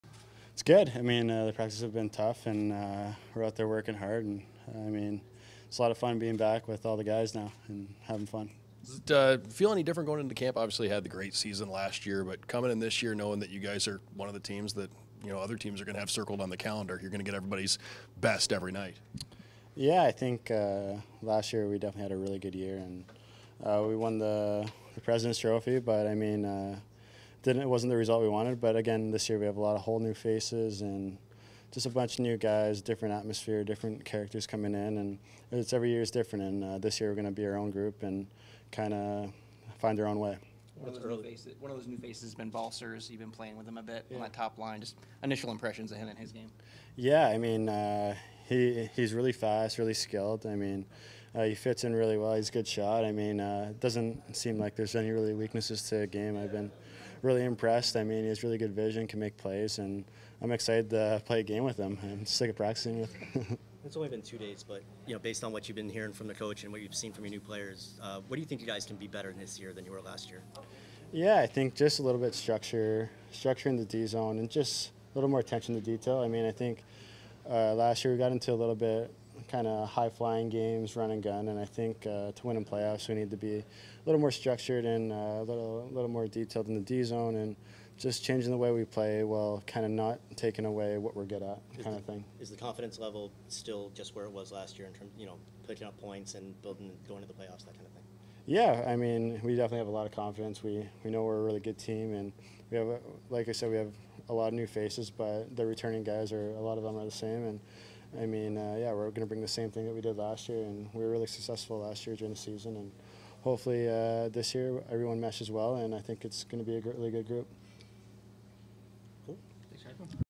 Carter Verhaeghe, Florida Panthers: Training Camp, Day 2
Florida Panthers forward Carter Verhaeghe speaks following the second day of 2022-23 training camp on Friday, Sept. 23 at the IceDen in Coral Springs.

Thank you all for the support all season long!